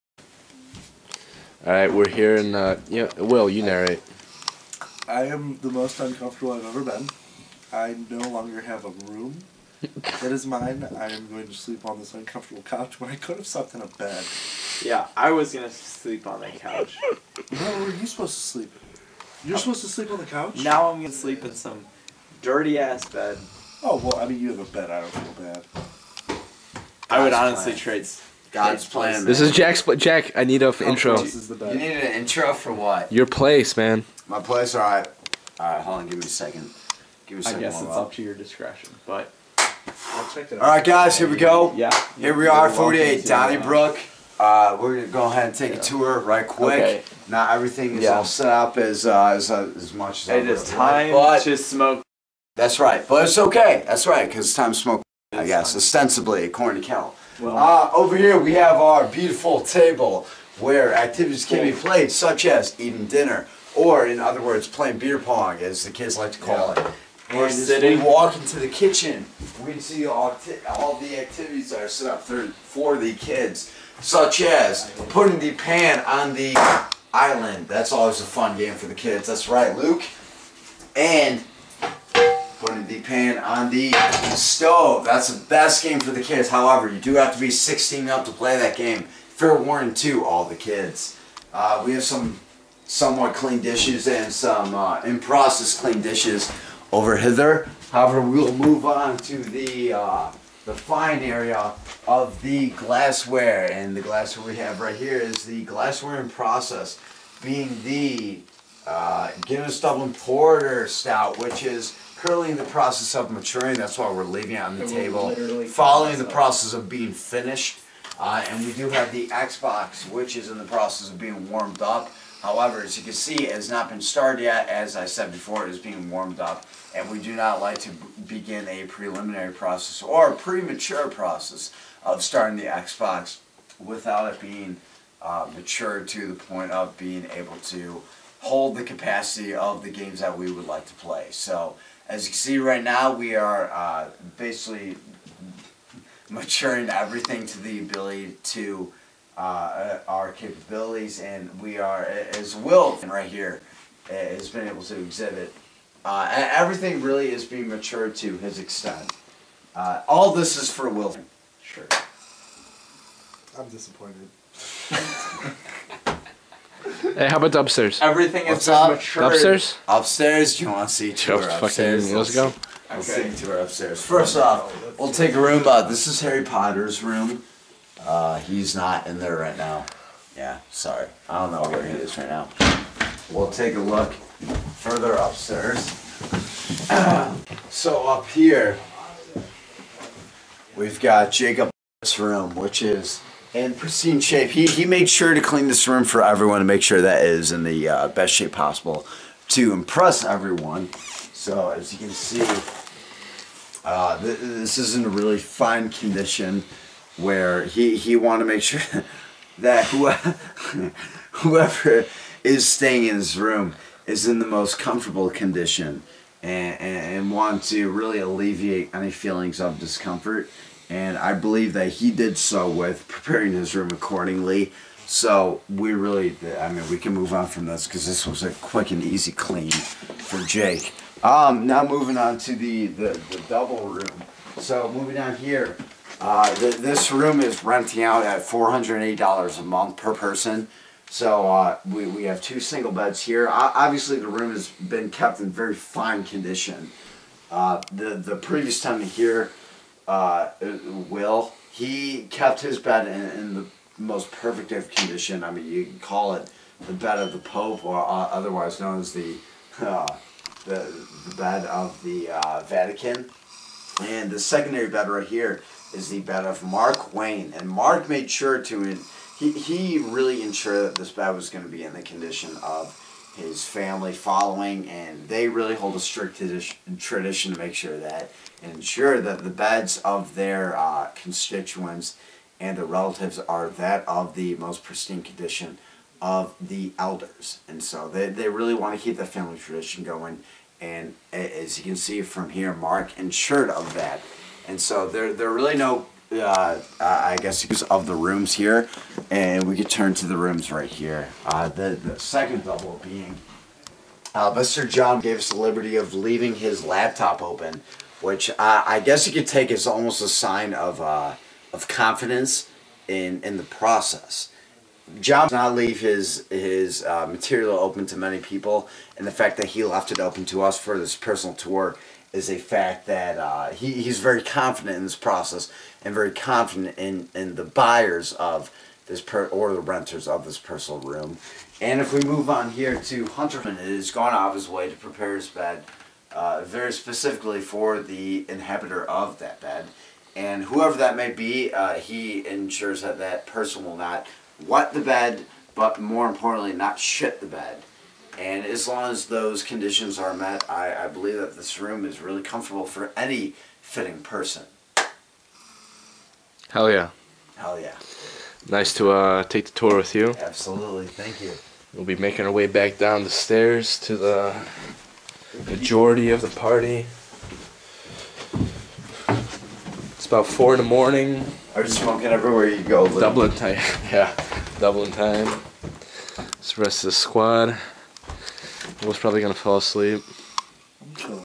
An Exclusive Tour of Jack's Southside Dublin Residence (Dublin part 3)
This is part 3 of our adventure in Ireland. After taking on the famous Temple bar, we found our way to Jack's temporary home (while he is studying abroad). After a long night of drinking Irish pints, the next best course of action was to enjoy an exclusive house tour given by none other than Jack, at 4 in the morning.